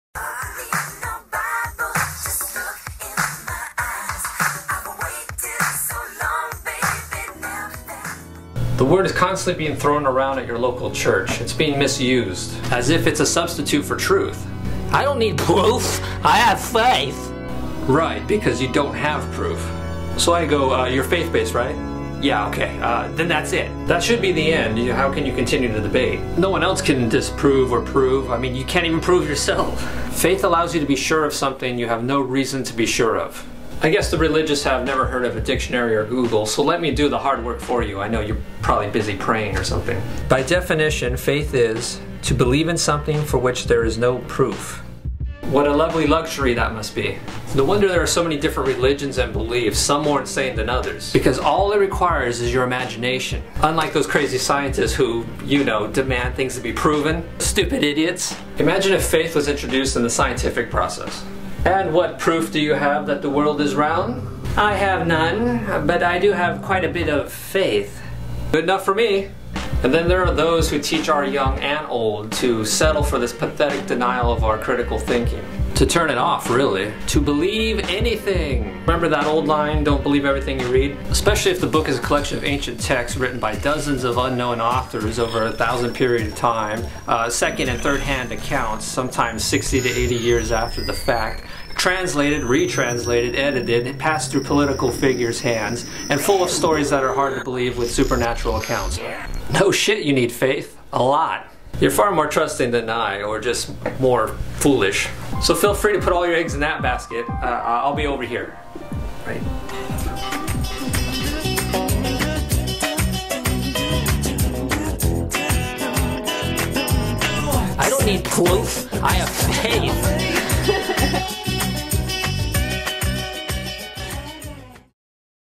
F@CK YOUR FAITH - The Dirty Atheist
The Dirty Atheist presents F@CK YOUR FAITH
A conversation regarding the meaning of faith.
Faith is not a virtue, it's embarrassing folks!